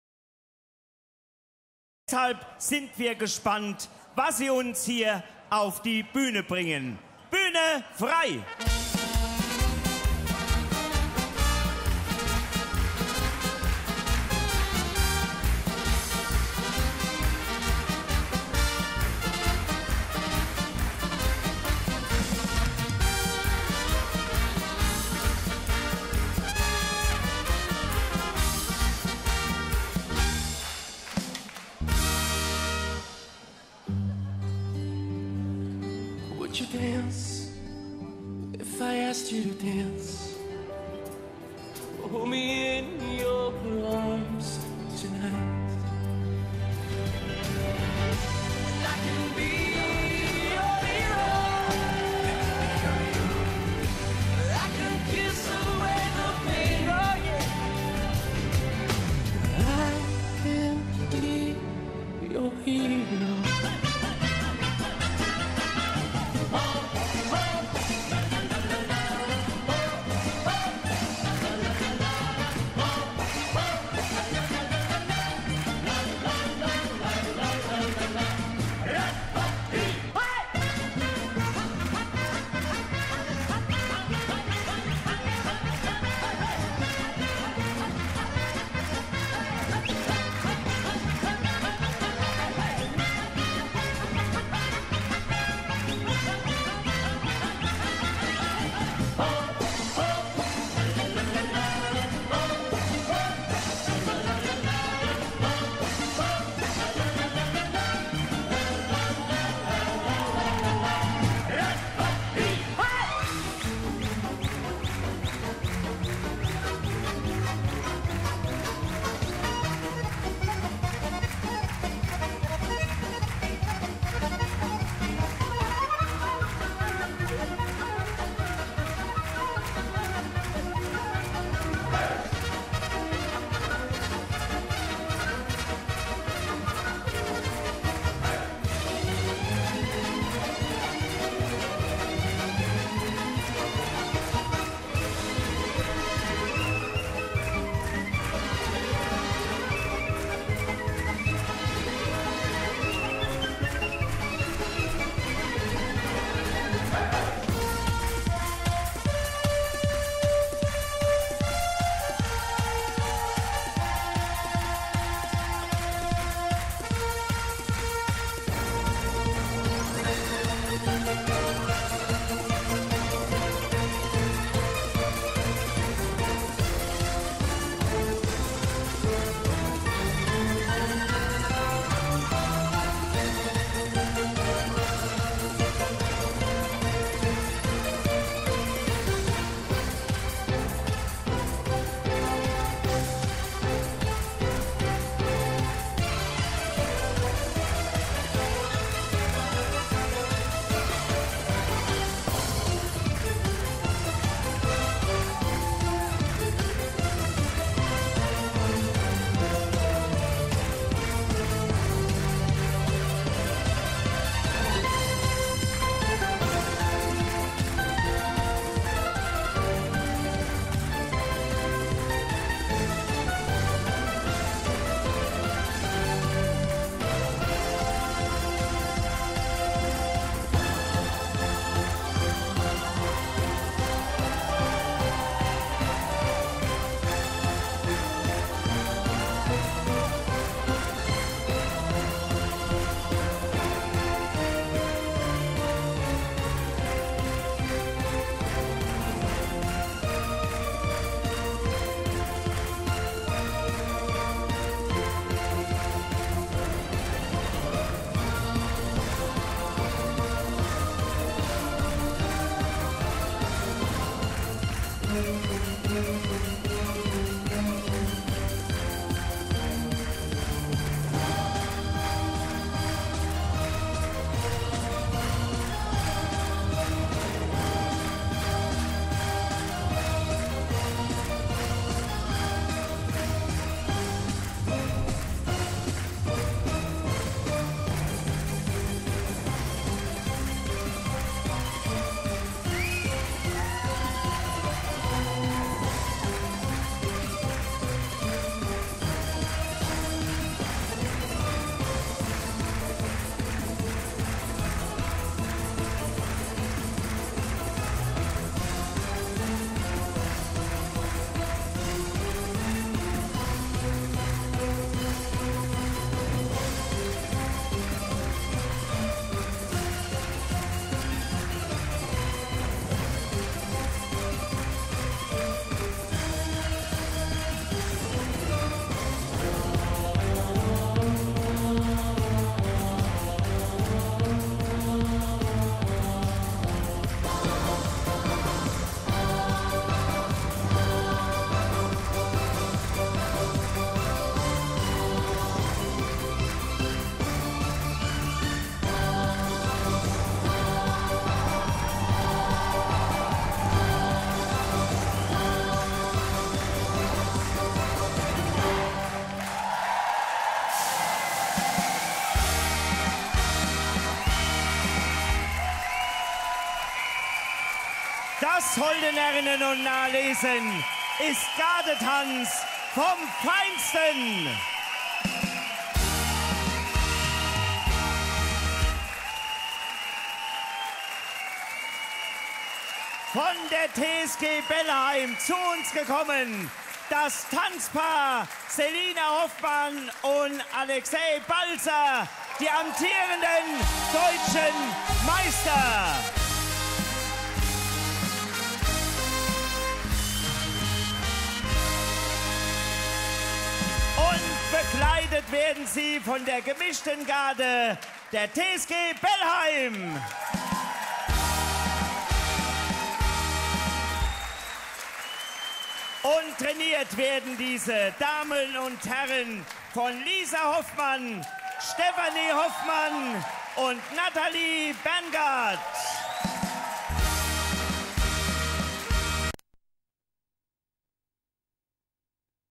Selina Hoffmann und Alexej Balzer und Gemischte Garde TSG Bellheim 2013/2014
Selina Hoffmann und Alexej Balzer und Gemischte Garde TSG Bellheim in einem Medley bei der Fernsehsitzung der Vereinigung Badisch-Pfälzischer Karnevalsvereine e.V.  2014